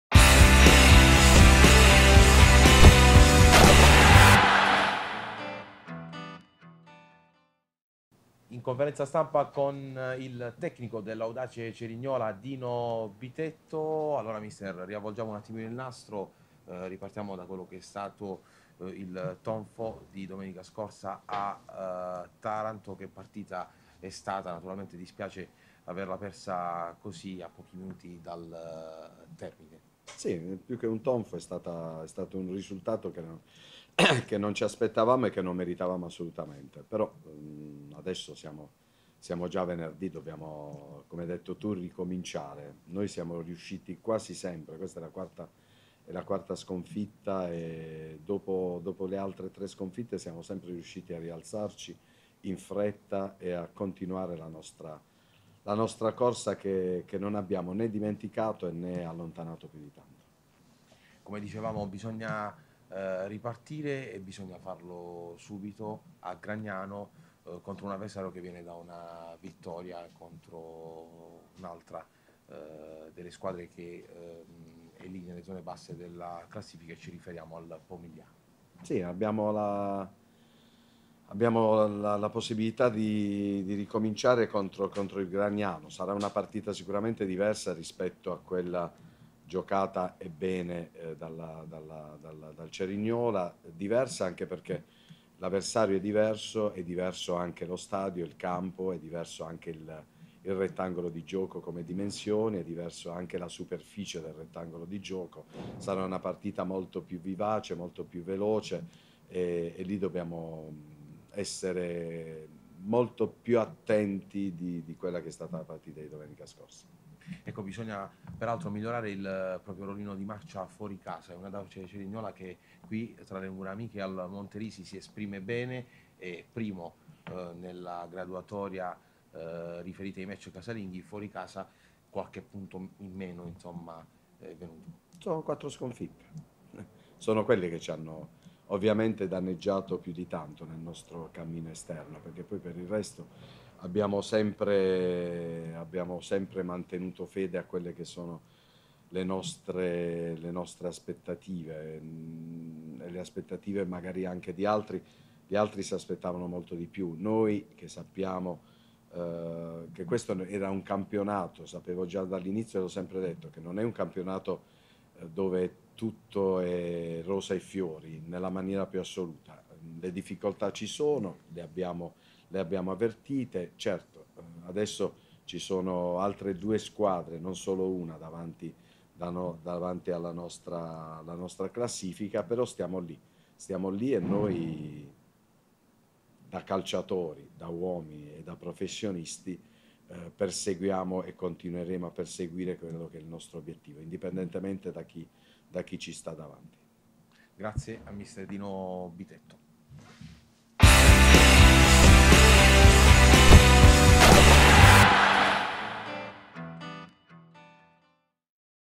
Francesco Zagaria intervista Dino Bitetto, tecnico dell'Audace Cerignola.
Il video è stato realizzato dall'Ufficio Stampa dell'Audace Cerignola